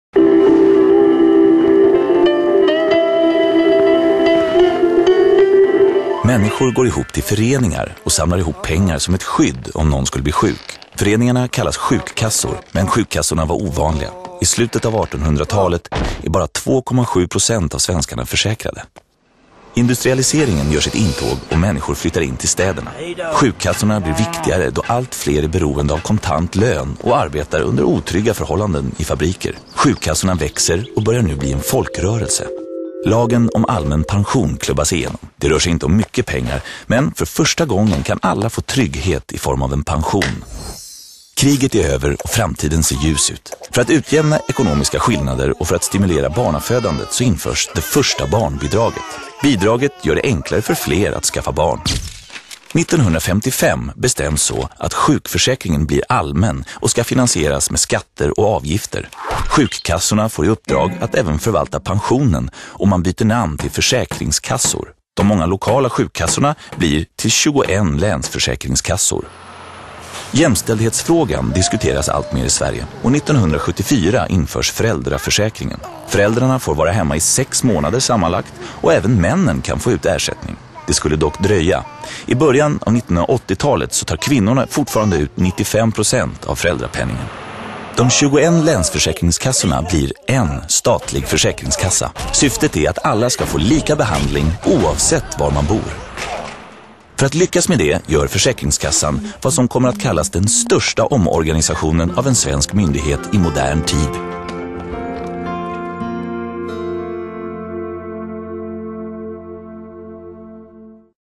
Socialförsäkringens historia
Socialförsäkringen är en viktig del av det svenska trygghetssystemet. Den svenska socialförsäkringen gäller i stort sett alla som bor eller arbetar i Sverige. Den här filmen berättar om hur socialförsäkringen vuxit fram och sätter de olika förmånerna i sina tidsperspektiv.

Försäkringskassan ansvarar för en stor del av de offentliga trygghetssystemen i Sverige.